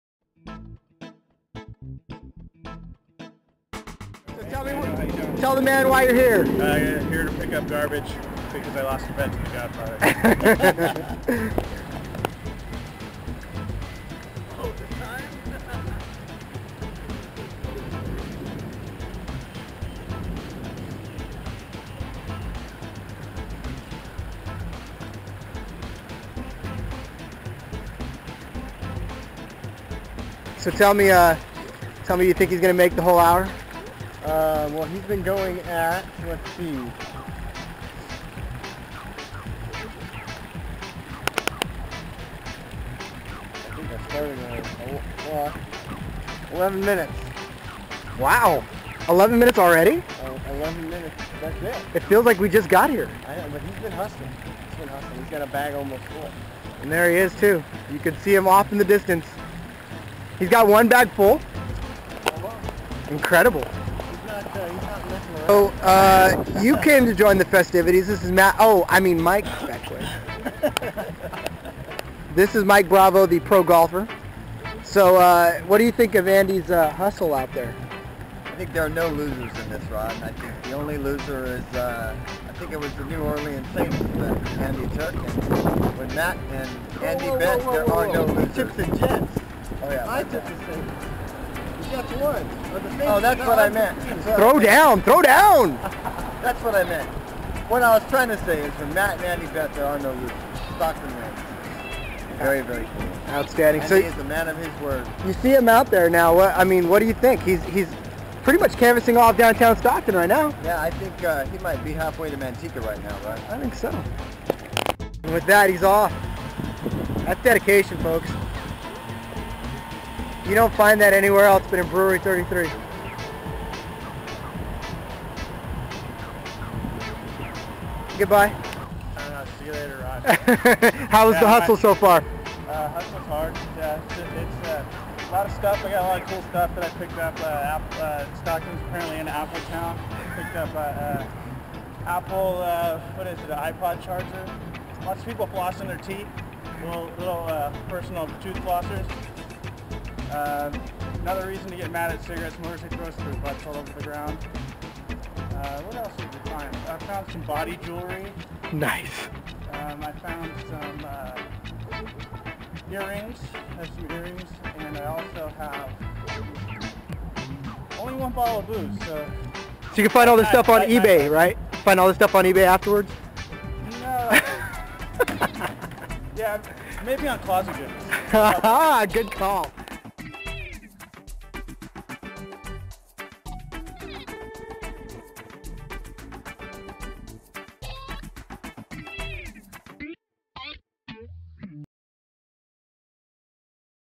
Trash bet 2010
Andy Pinasco (a.k.a Brewery33 on Twitter) settles his Super Bowl "trash bet" with Matt. His payment, civic duty in the form of collecting trash for one hour in Downtown Stockton. Podcast Stockton cameras were the to catch the event.